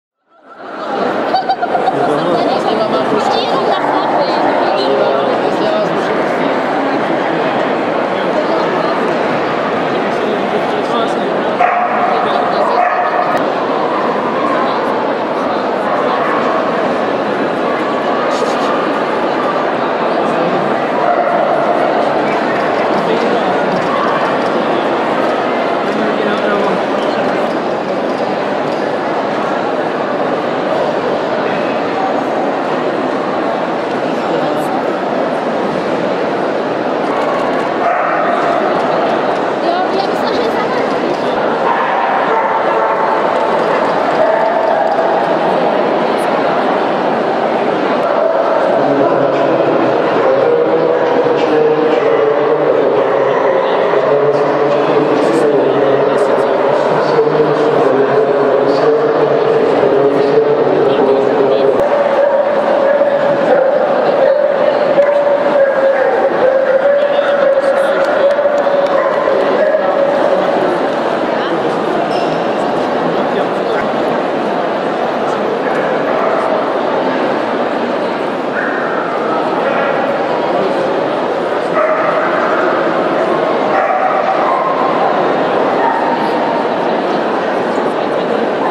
Vega Clark, CAC, CACIB, BOB [MVP 05.11.2011]
Vega Clark získala na Mezinárodní výstavě v Praze (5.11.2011) tituly CAC, CACIB a BOB - vítěz plemene